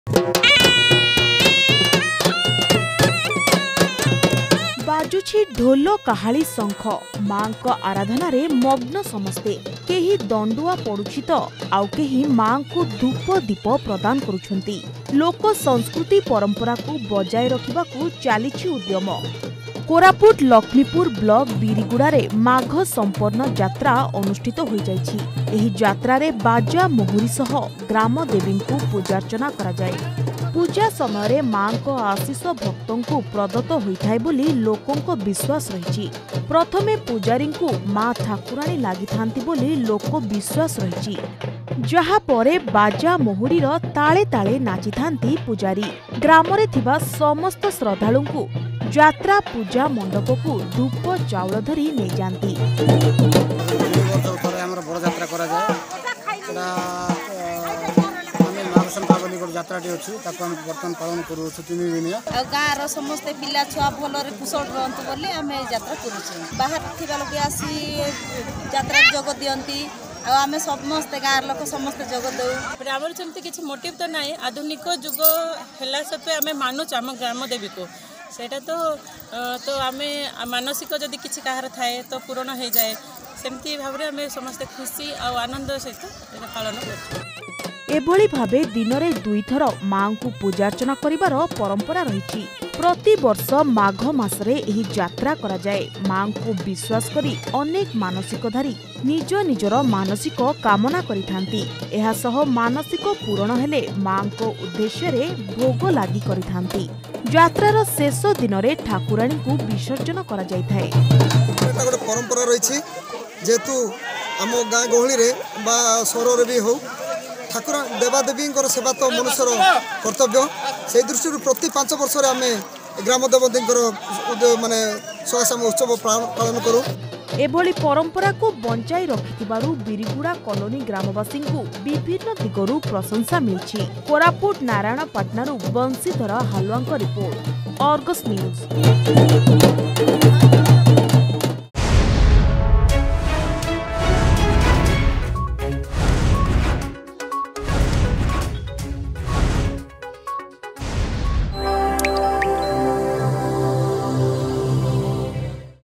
ମାଙ୍କ ଆାରାଧନାରେ ମଗ୍ନ ସମସ୍ତେ
ପୁରାତନ ସଂସ୍କୃତି ଓ ପରମ୍ପରାକୁ ବଜାଇ ରଖିବାକୁ ଠାକୁରାଣୀ ଓ ଗ୍ରାମଦେବୀଙ୍କ ଆରାଧାନା । କୋରାପୁଟ ଲକ୍ଷ୍ମୀପୁର ବ୍ଲକ୍ ଅନ୍ତର୍ଗତ ବିରିଗୁଡା କଲୋନୀ ଗ୍ରାମବାସୀ ପ୍ରତି ତିନି ବର୍ଷରେ ଥରେ ମାଘସମ୍ପା ଯାତ୍ରା ପାଳନ କରିଥାନ୍ତି.